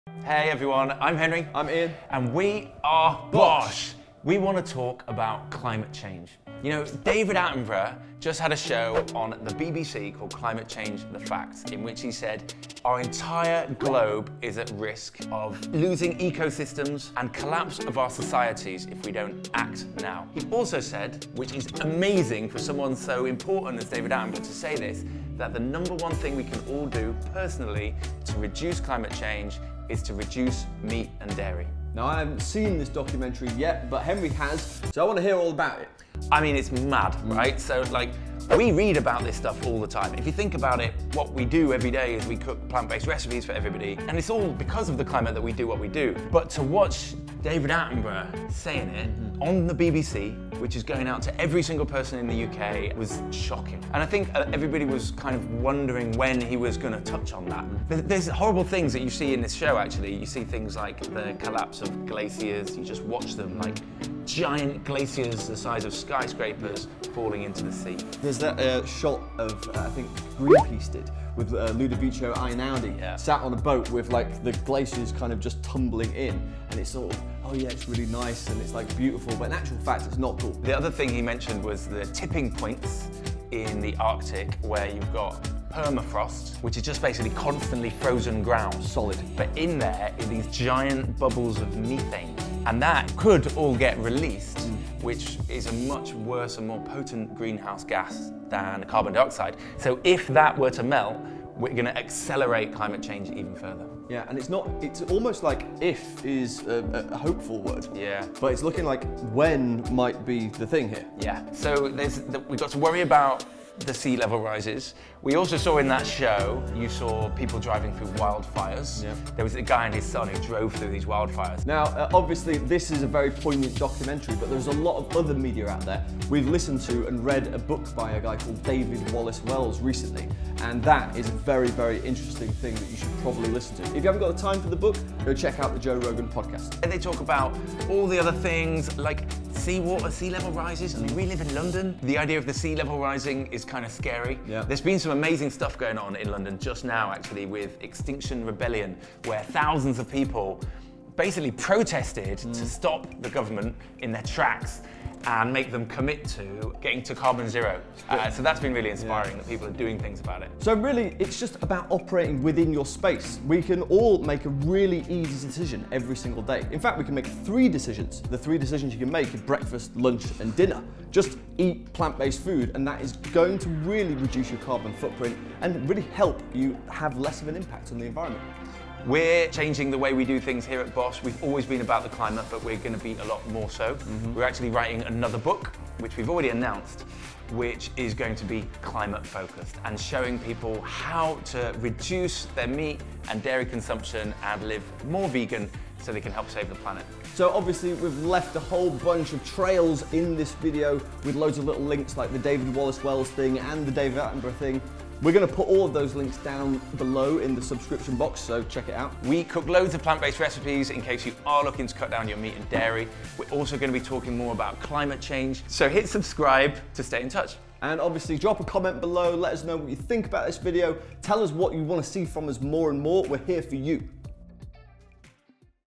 ARE YOU WORRIED ABOUT CLIMATE CHANGE? 🌍
Have you guys seen ‘Climate Change - The Facts’ yet?

Another absolutely brilliant show with the legendary David Attenborough. It was amazing to hear on the BBC that the most important thing we can all do as individuals is to reduce our meat & dairy consumption!

Climate change has been a huge topic of discussion lately with both the protests in London from @exctinctionrebellion, activists like Greta Thunberg, and countless shows like “Our Planet”!

It has never been so important to help save our amazing planet.

If you’re looking to cut down on your meat and dairy consumption, check out our recipes, as well as all the other delicious plant-based channels out there!

All the love, Henry & Ian

★ SIGN UP TO RECEIVE FIVE BITS OF FOODY FUN EVERY FRIDAY then go to SIGN ME UP